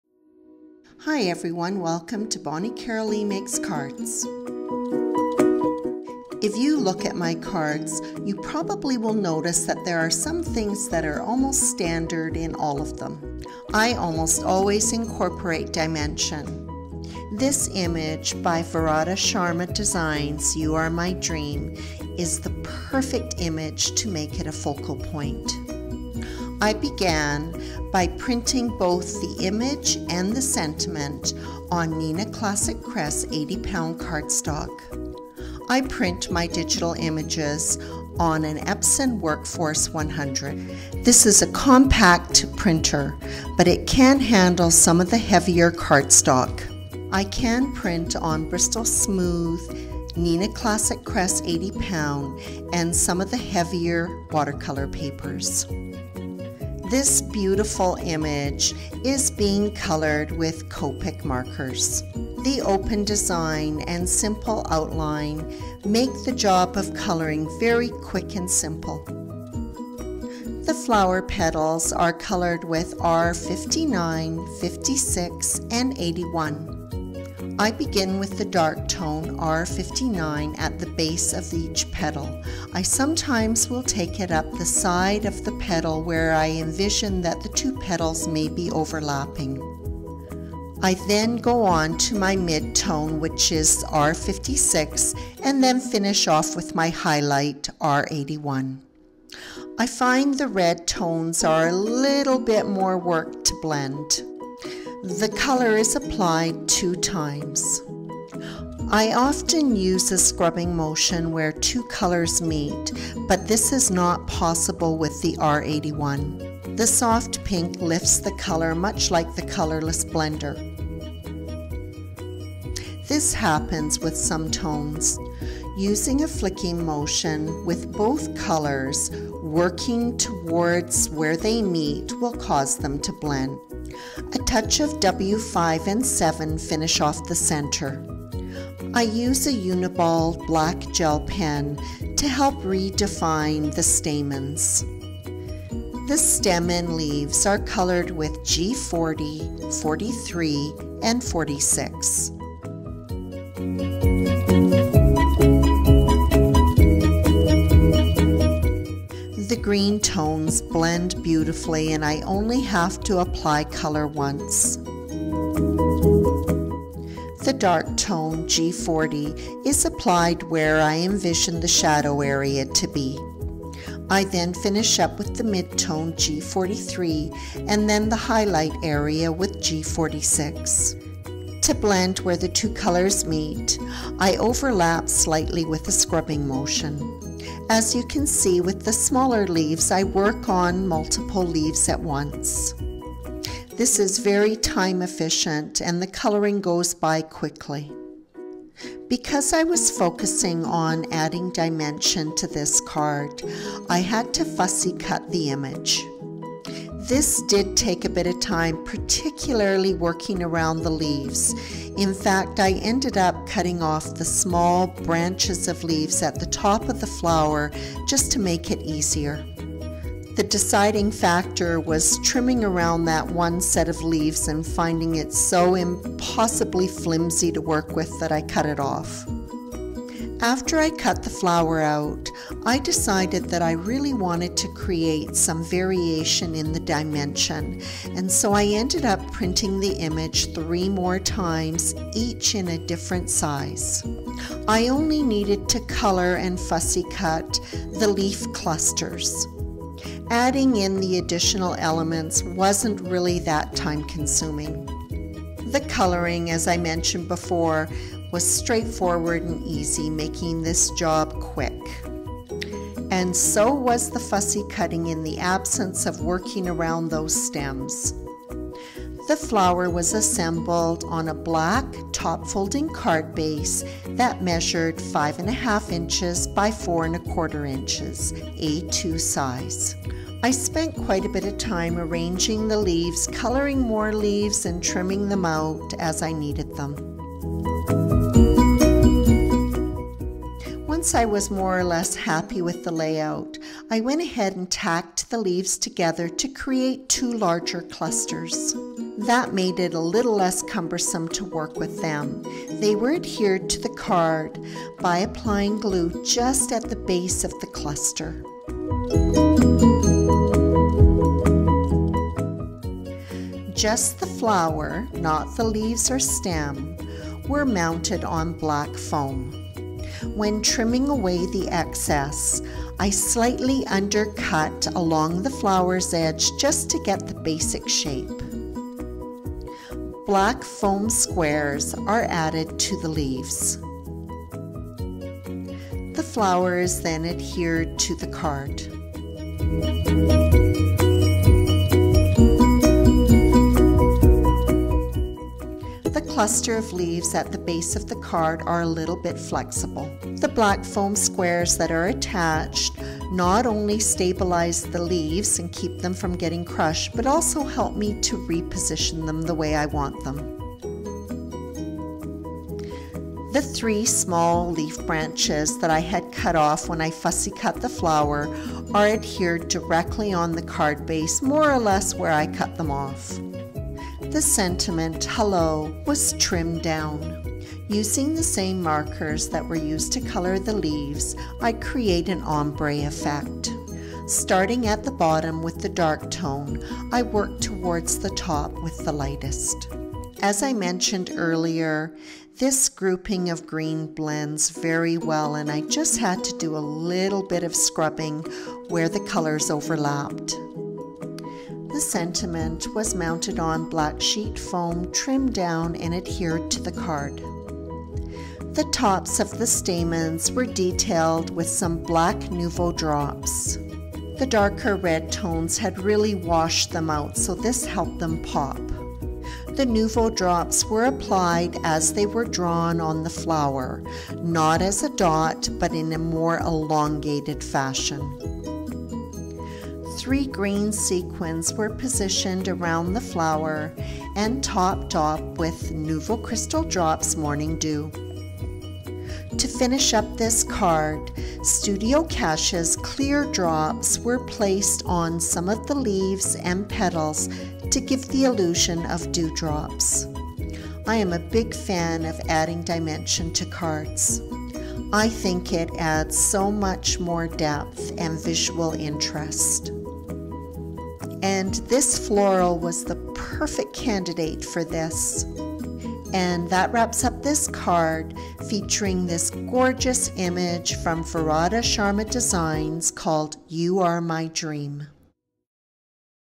You Are A Dream … creating dimension
Hello everyone! I have another card from Varada Sharma Design’s You Are Beautiful release featuring You Are A Dream. This digital stamp set includes a gorgeous floral and many supporting sentiments. I almost always include dimension in my cards and decided I would make this a focal point for this card.